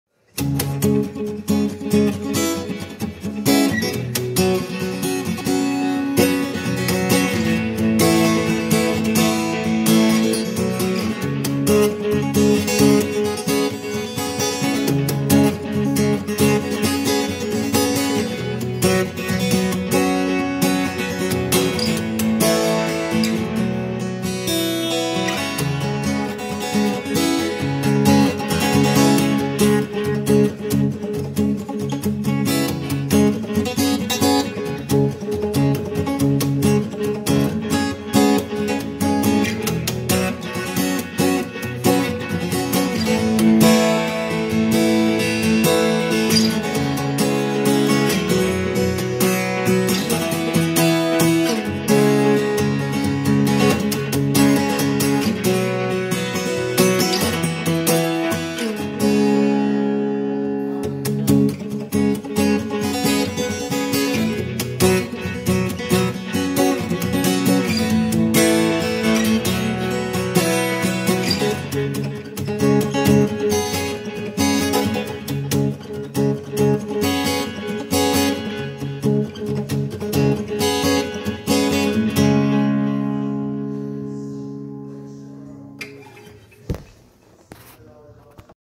Music store improv 2021
"Here, try this.."  I was handed a Kepma guitar at Music Villa, O WOW 😊   I don't know why the video got blurry at the end...enjoy!